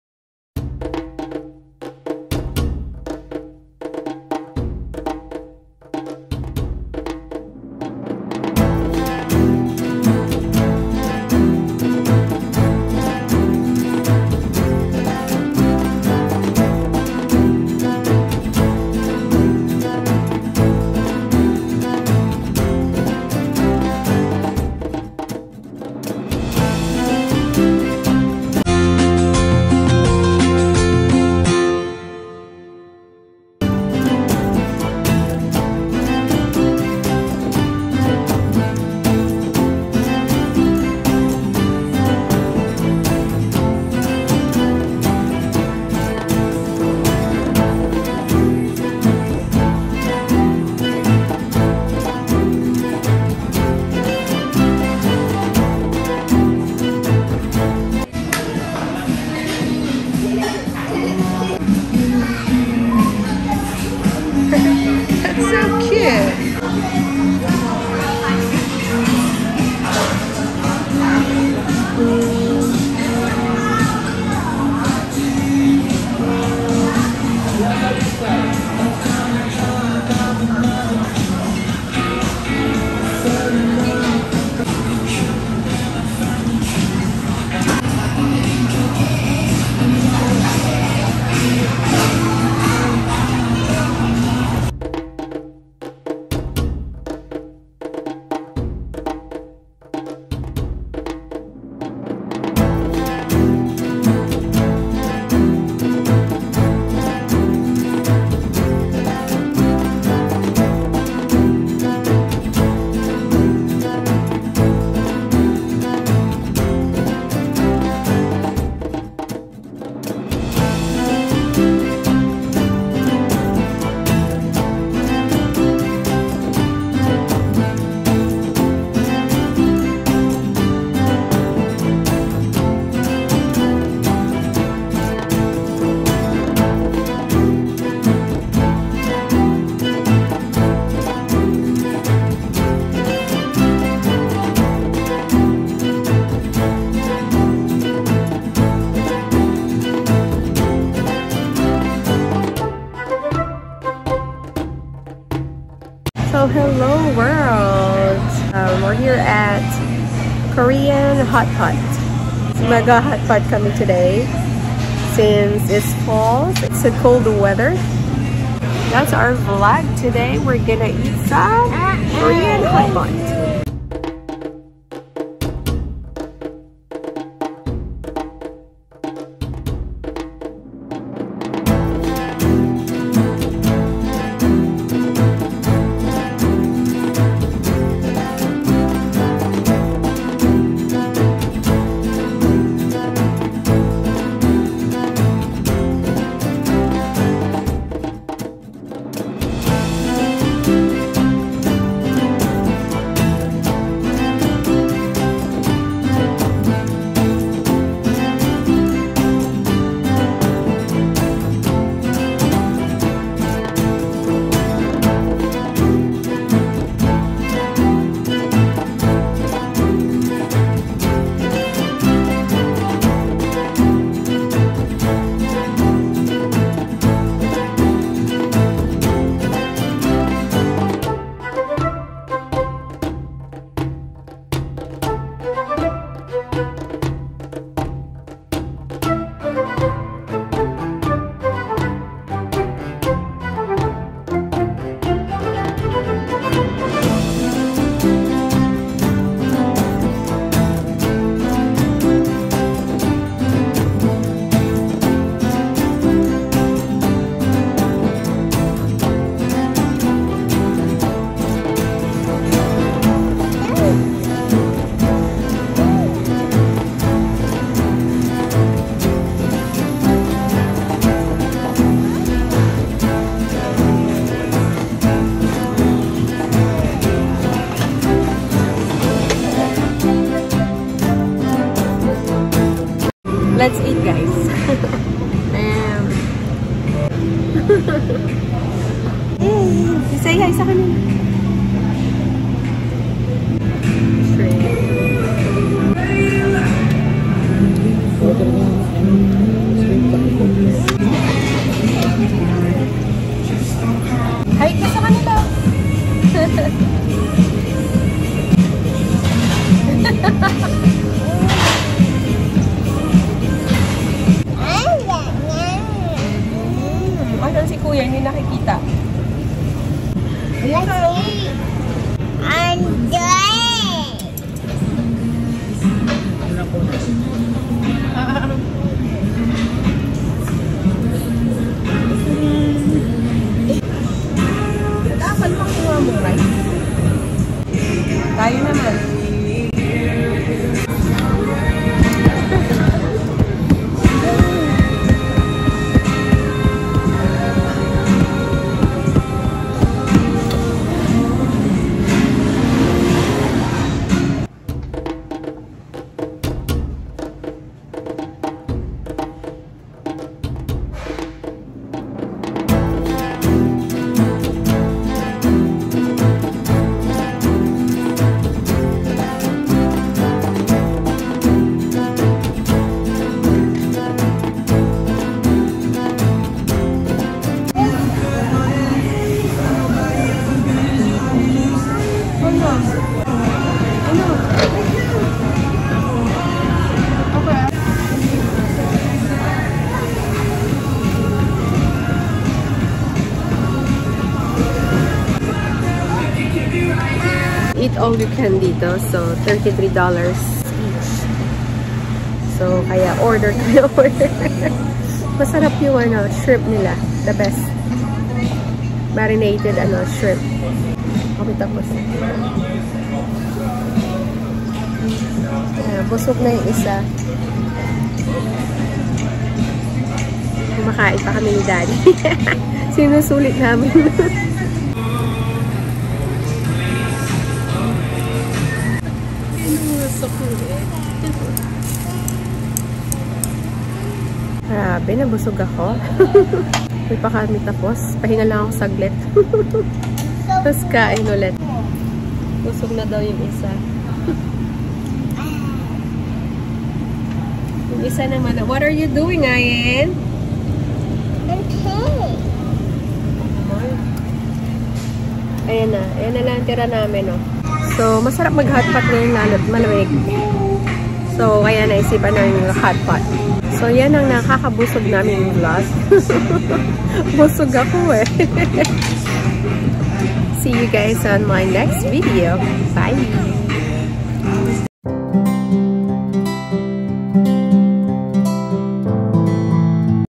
Korean Hotpot & Barbecue At Fremont California | annasworld
Hotpot is a perfect for cold weather. We’re also craving for Korean food so we decided to go to this Korean Hotpot & barbecue.